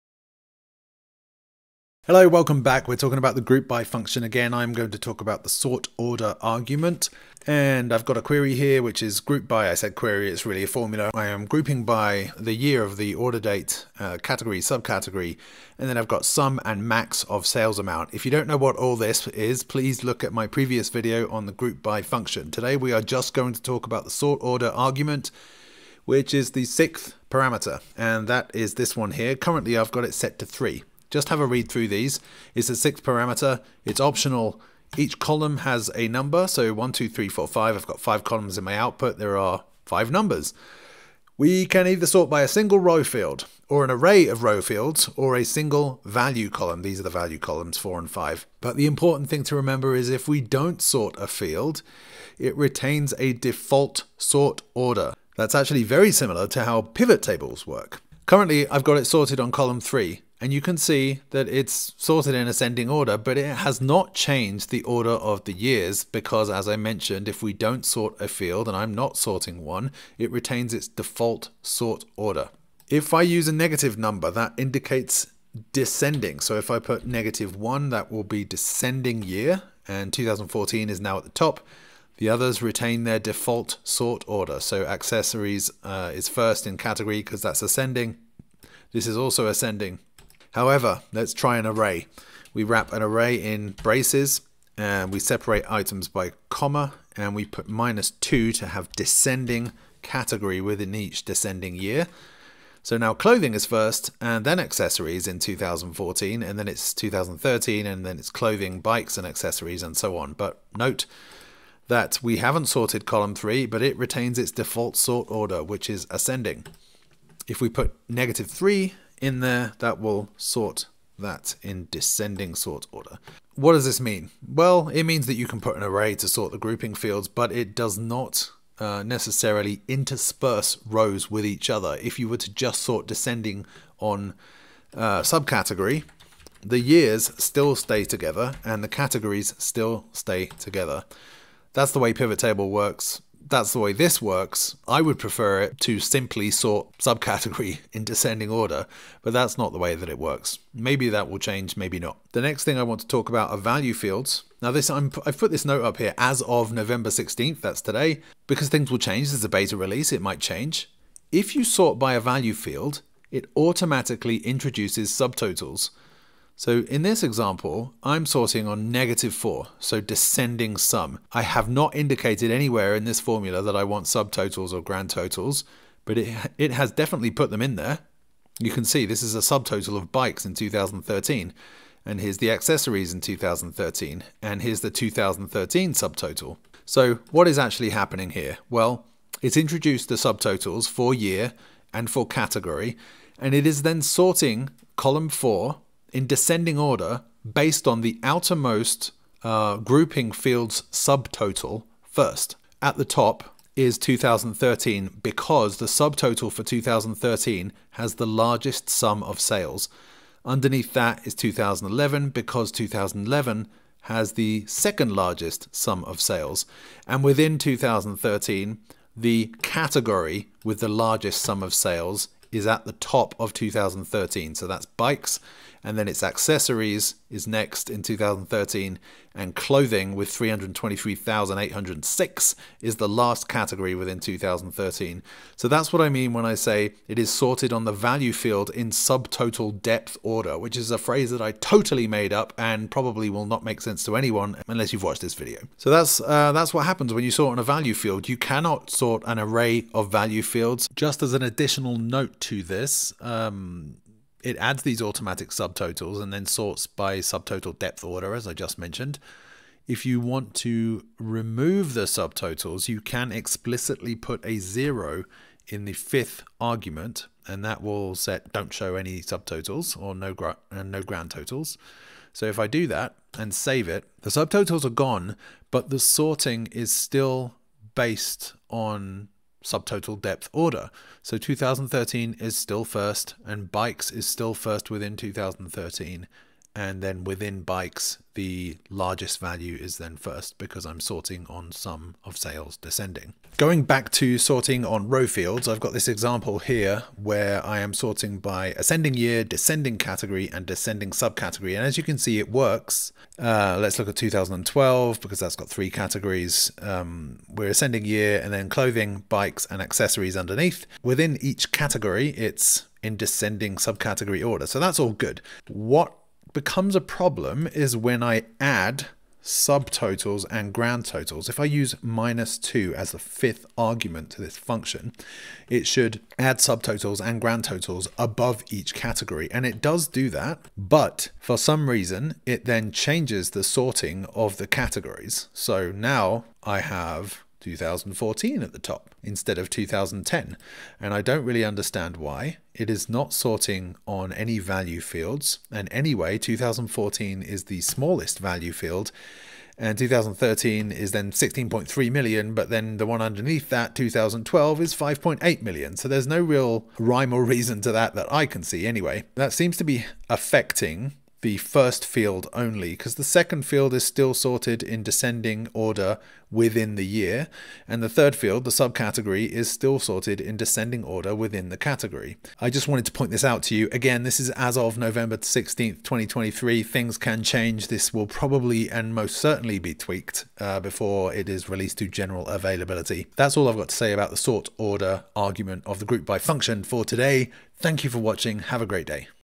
More on Excel's NEW GROUPBY function - the sort_order parameter
This brand new GROUPBY function is a huge release for Excel - and it's a function that does A LOT. The sort_order parameter is perhaps not straightforward, so in this video I talk about how it works, what you can do with it, what you can't do with it, and some perhaps counter-intuitive behavior I've seen as of November 16 2023.